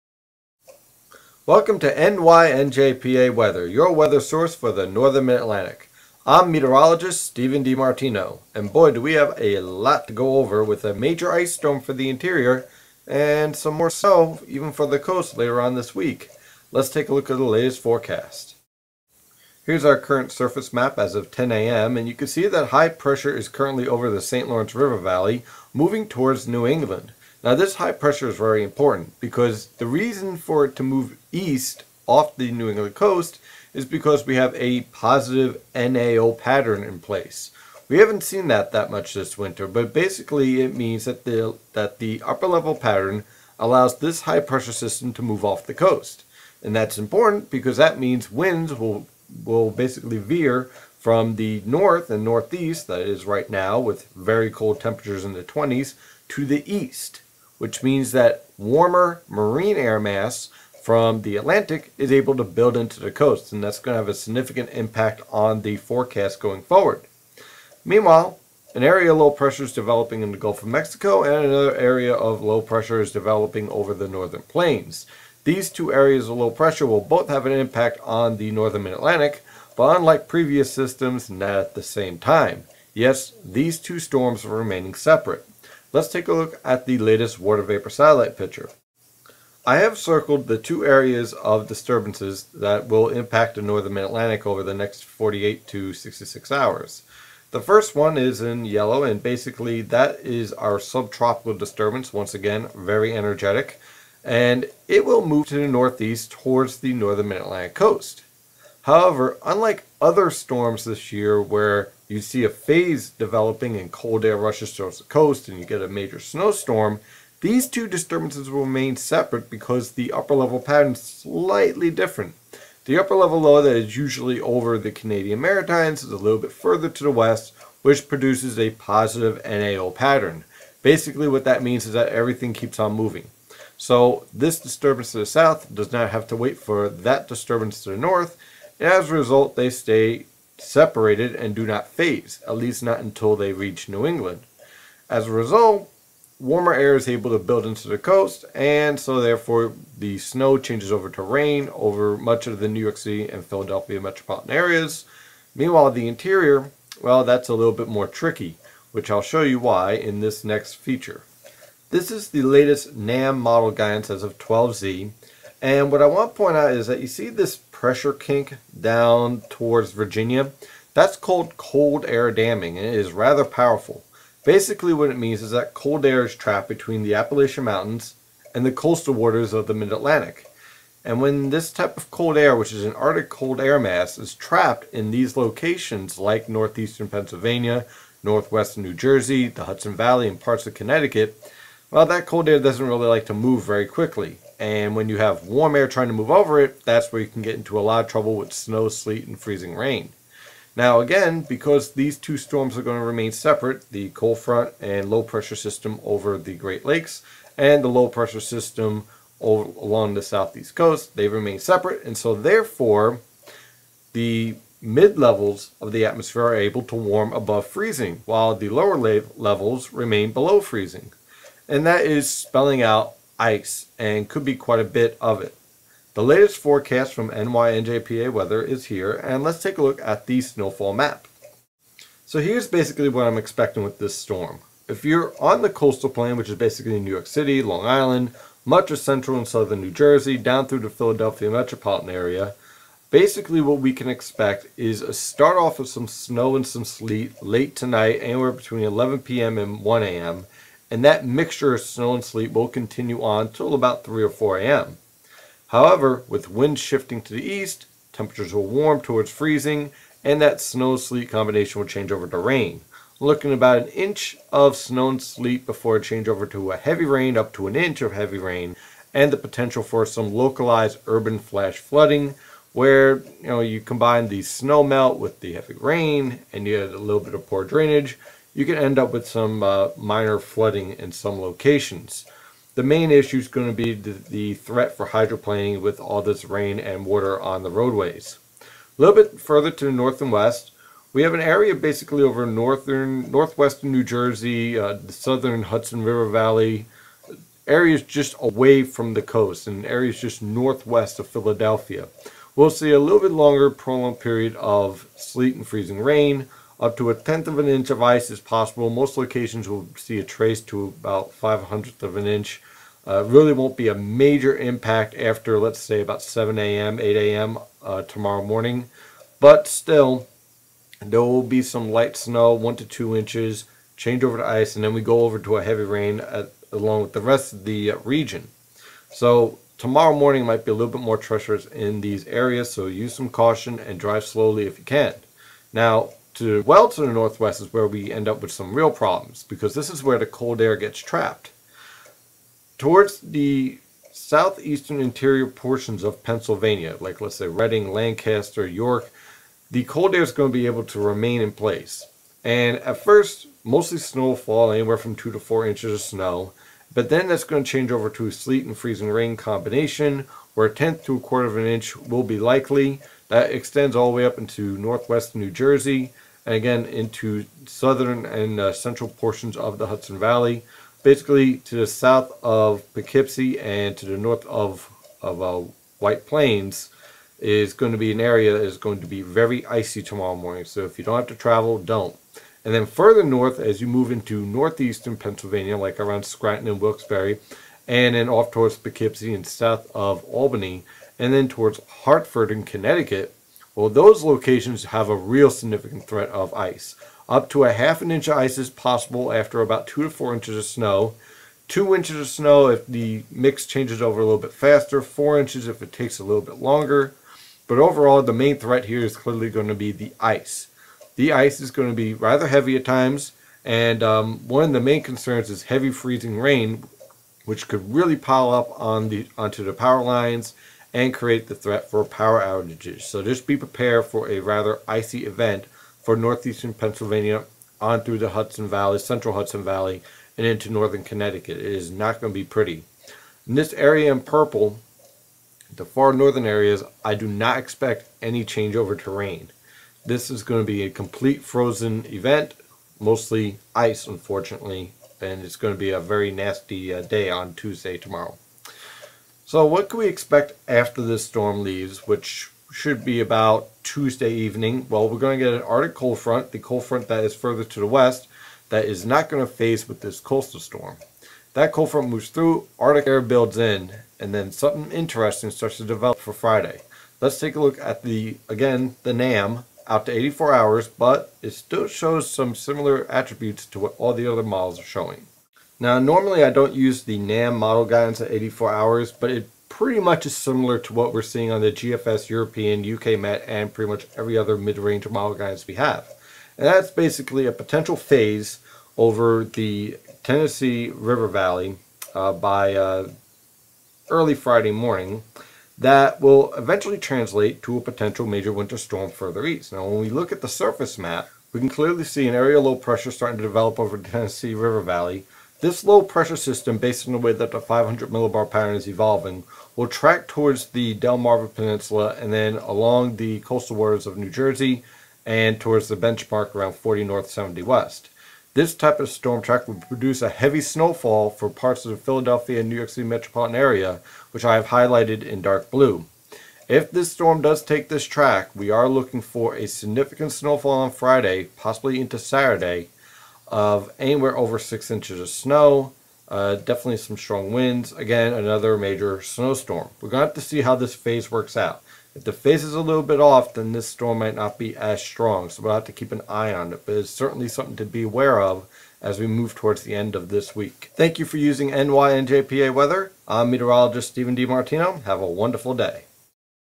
Forecast Discussion for January 17, 2011
An ice storm is setting up for the interior while heavy rain brings the threat for localized flooding for New York City and Philadelphia.  Plus a look at another major winter storm for the end of this week!